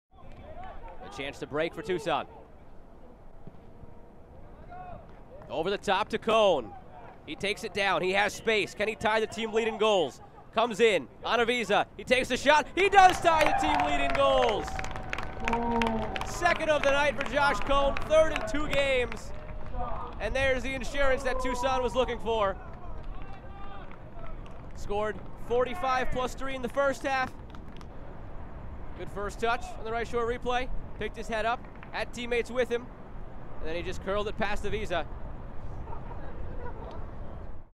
Joshua Coan with a Goal vs. Orlando City B
Joshua Coan (FC Tucson) with a Goal vs. Orlando City B, 10/17/2020